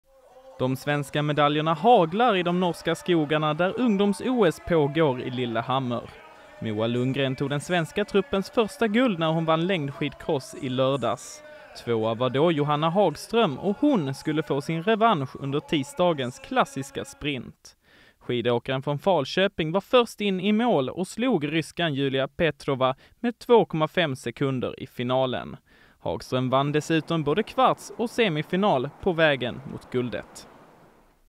Johanna Hagström tog guld vid ungdoms-OS
De svenska framgångarna fortsätter i Lillehammer och ungdoms-OS. Under tisdagen kom ett nytt guld när Johanna Hagström krossade allt motstånd i den klassiska sprinten.